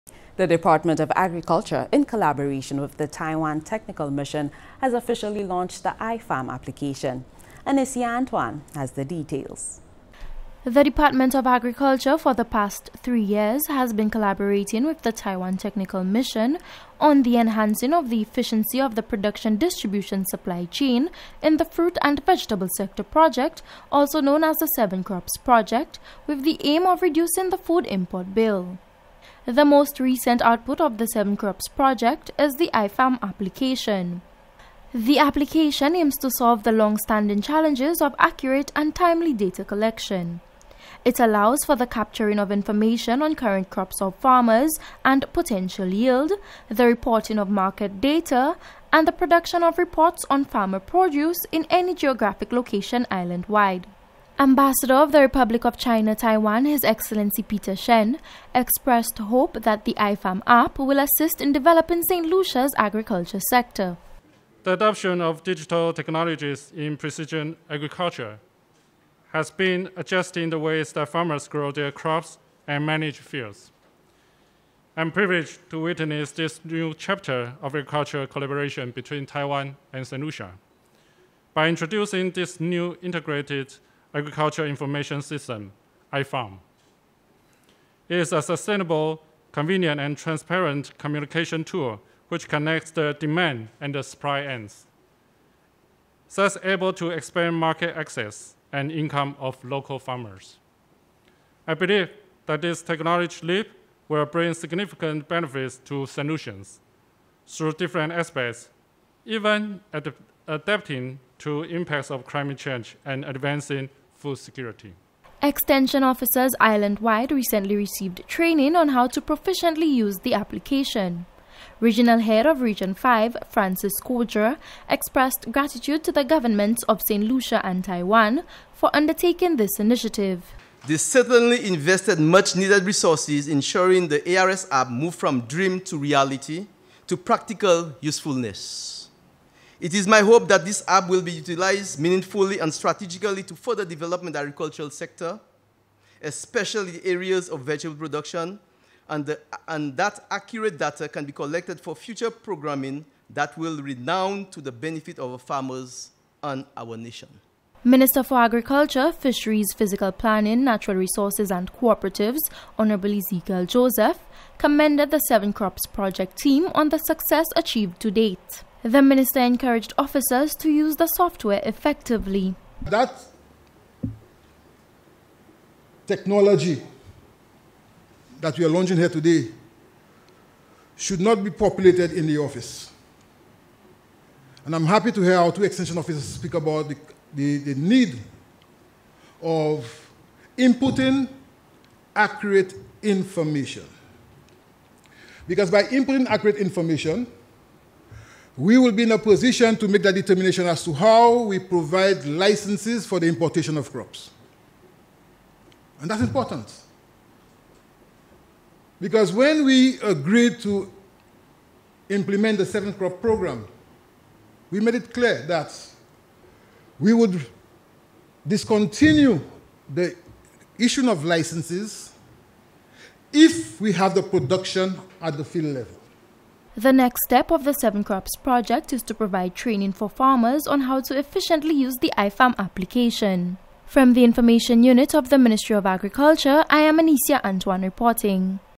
IFarm App Launched
The Department of Agriculture in collaboration with the Taiwan Technical
Mission has officially launched the iFarm application.